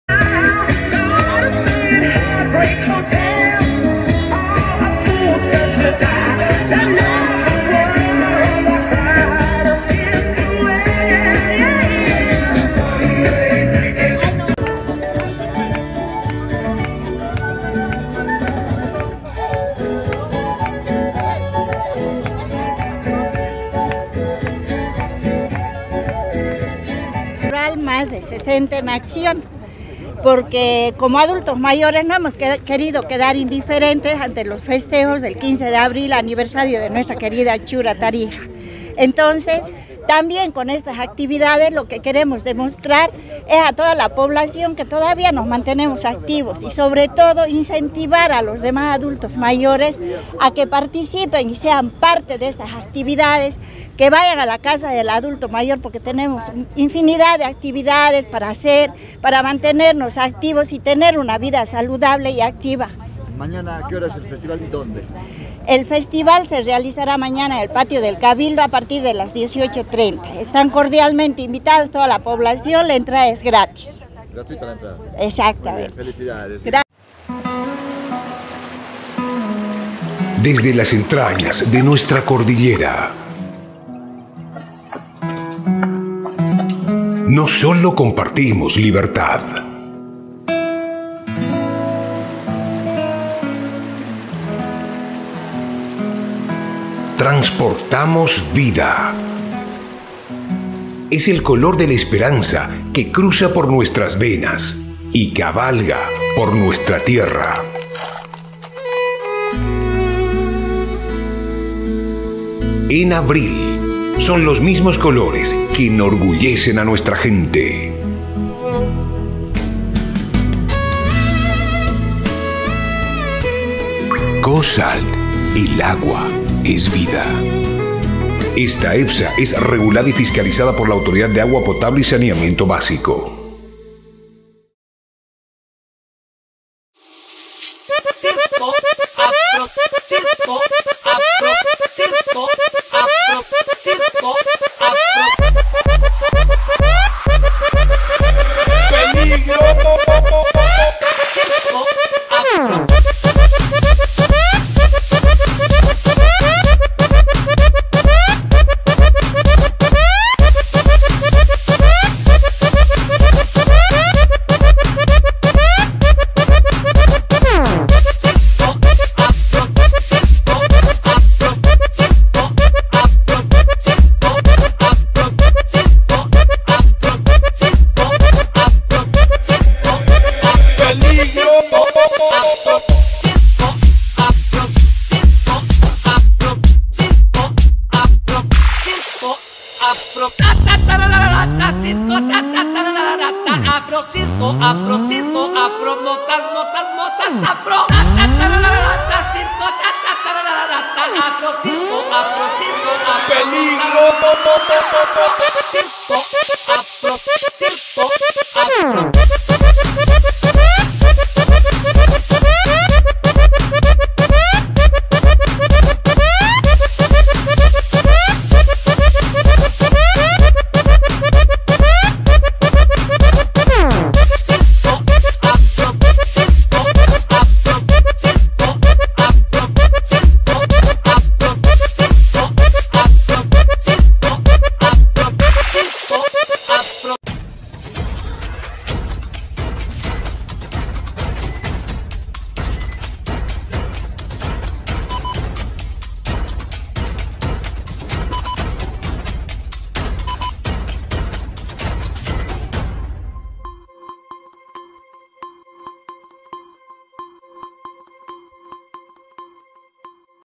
"FESTIVAL CULTURAL MÁS DE 60 EN ACCIÓN" A REALIZARSE EL MIÉRCOLES 26 DE ABRIL
SE INVITA A LA POBLACIÓN AL "FESTIVAL CULTURAL MÁS DE 60 EN ACCIÓN" A REALIZARSE EL MIÉRCOLES 26 DE ABRIL, EN EL PATIO DEL CABILDO A PARTIR DE LAS 18:00